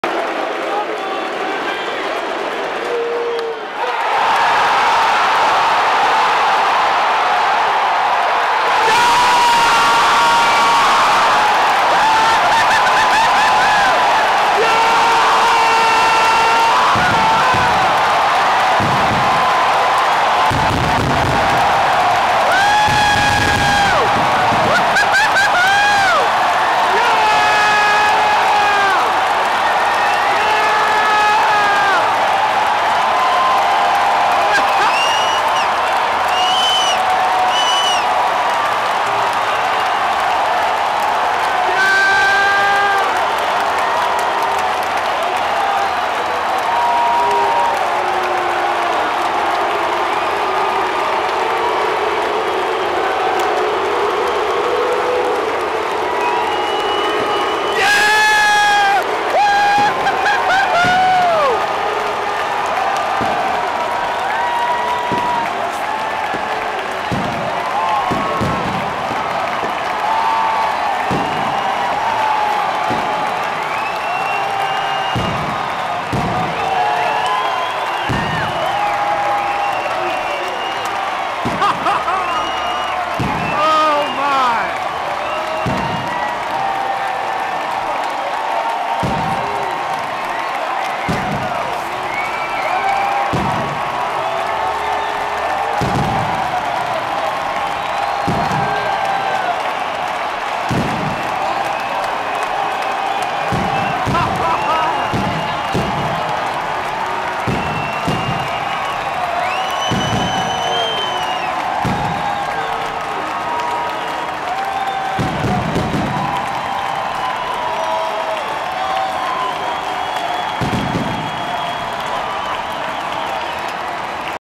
Jay Bruce Walk Off HR for Reds and the Celebration That Ensued
Luckily, I switched my camera to video mode last night in hopes of catching something special. That's exactly what happened. A few seconds after starting to record, Jay Bruce ended a 15-year playoff drought for the Reds and sent the city, the stadium, and clearly myself into party mode. This was the greatest moment in my life as a Reds fan, and a night I will never forget.